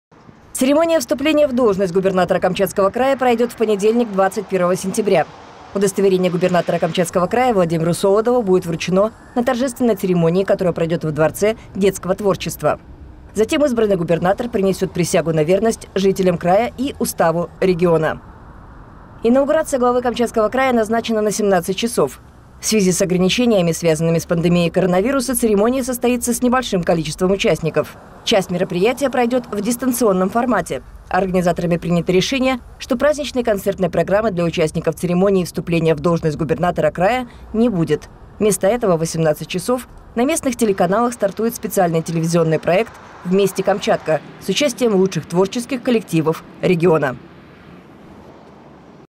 Инаугурация Солодова
Церемония вступления в должность Губернатора Камчатского края пройдёт в понедельник, 21 сентября. Удостоверение губернатора Камчатского края Владимиру Солодову будет вручено на торжественной церемонии, которая пройдет во Дворце детского творчества. Затем избранный губернатор принесёт присягу на верность жителям края и Уставу региона.